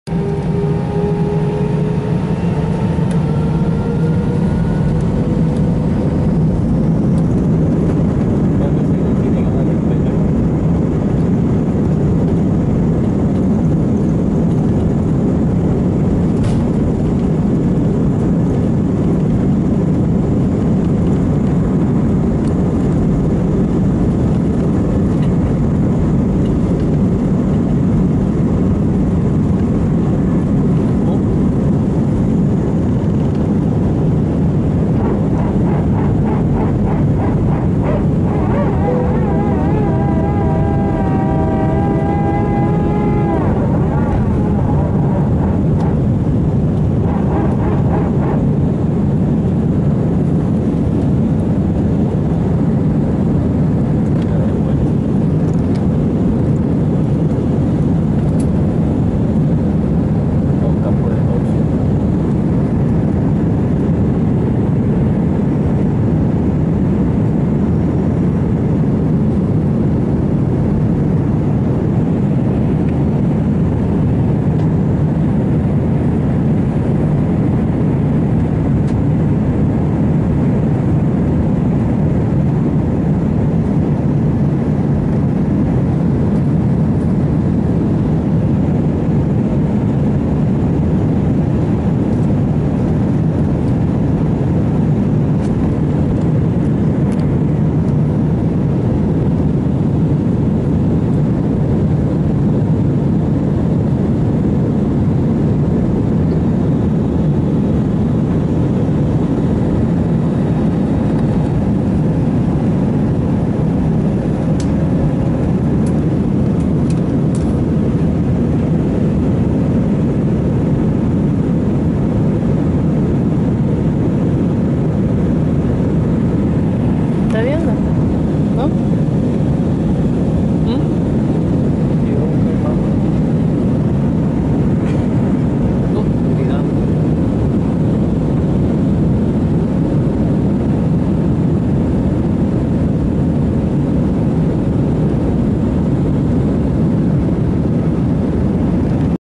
Despegando De Punta Cana a Puerto rico
Un A320  despega de Santo Domingo